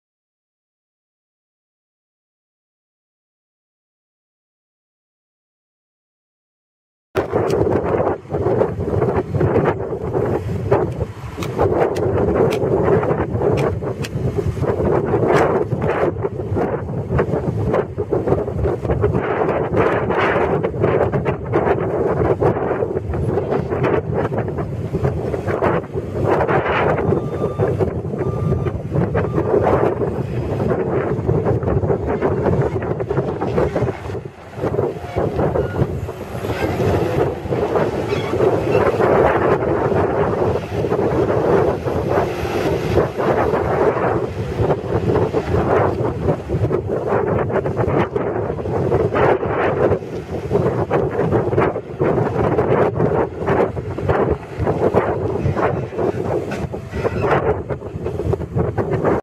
2011 Case SR175 skid steer for sale | no-reserve Internet auction July 20, 2017
2011 Case SR175 skid steer
993 hours on meter
Shibaura four cylinder turbo diesel engine
Serial 91484
Hydrostatic transmission
Joystick controls
Spring suspension seat
Backup alarm
71"W low profile bucket
14.8 cu.ft. capacity
Auxiliary hydraulics
Upper left headlight cluster will be replaced
Serial JAFSR175TBM428180
It's located in Bunker Hill, KS, and we're selling it to the highest bidder regardless of price.